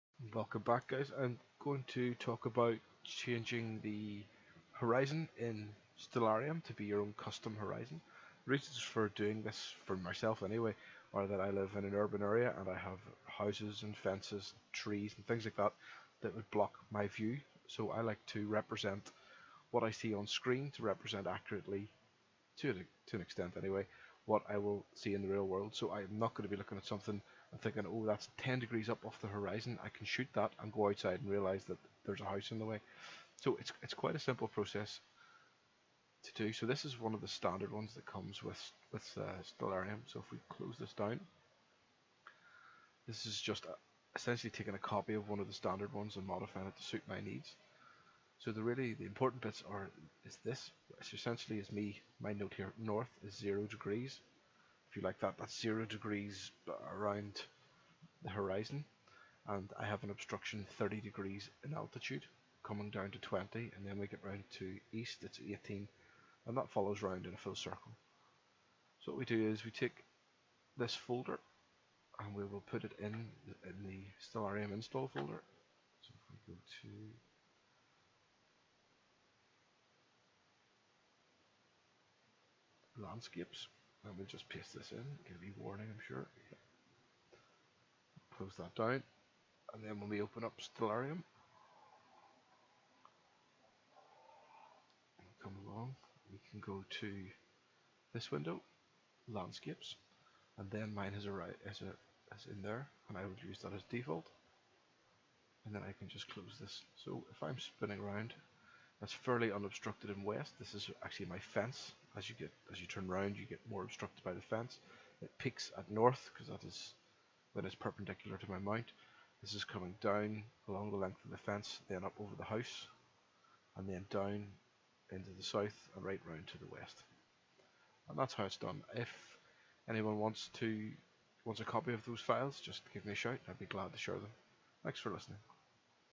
How to: Stellarium - Custom Horizon
A short video showing how to create and apply a custom horizon in Stellarium